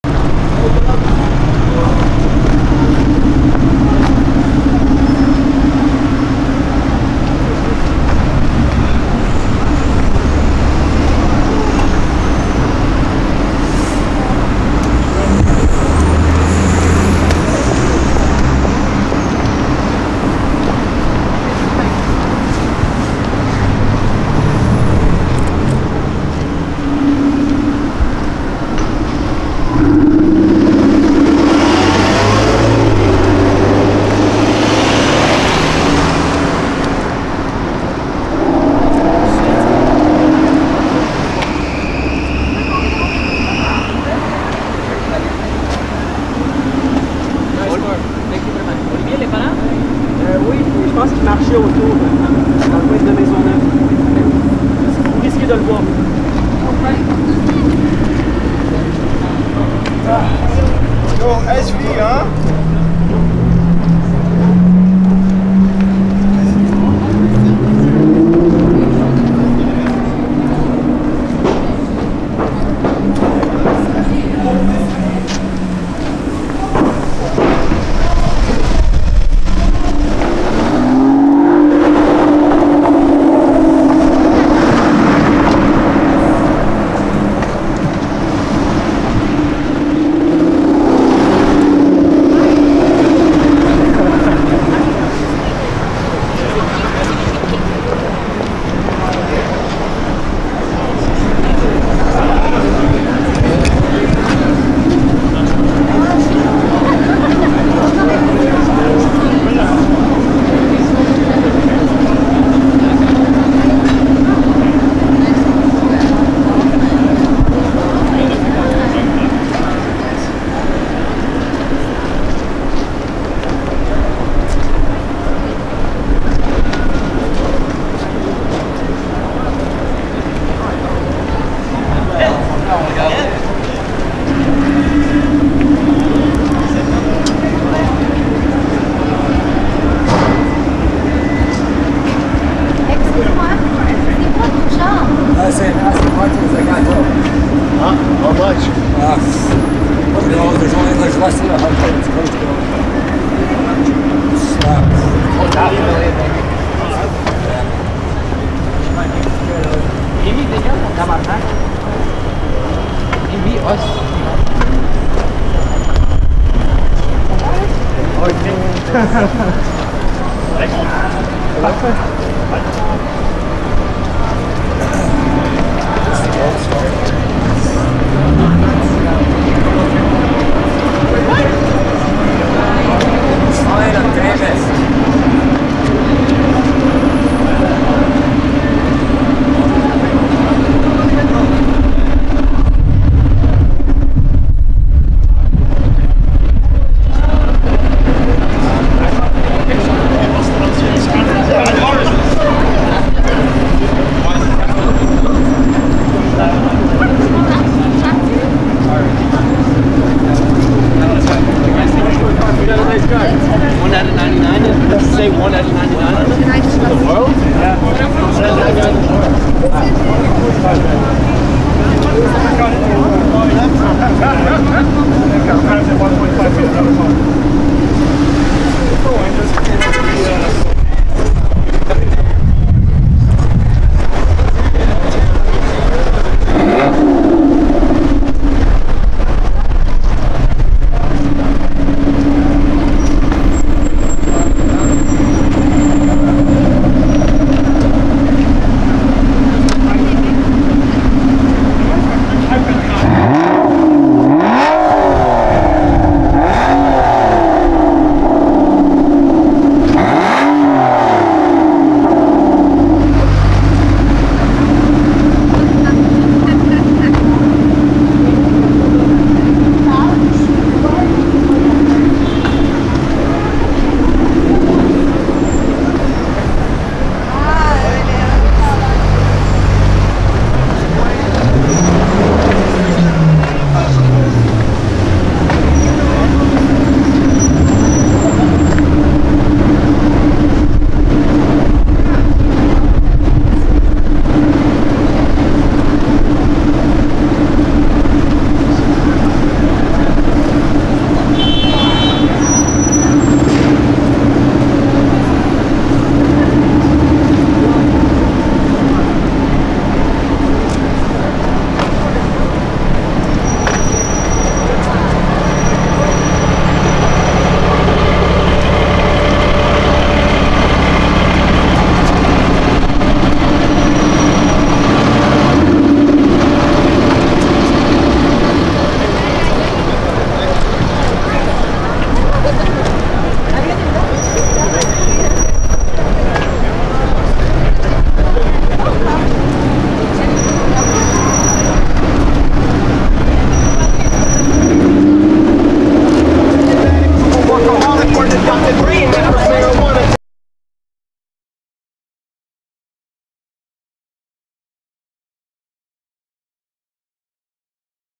The $1.5 MILLION Aston Martin Vanquish Zagato Shooting Brake
I was lucky Enough to get some good footage of 1 of 99 Aston Martin Vanquish Zagato Shooting Brake 1 of 2 in Canada and the only one in this colour.
Enjoy the beautiful music of this rare machine.

To see the pictures of the cars I come across check out our Instagram @Gamesta_TV

Livestreams and Weekly content with video games and lots of Supercars!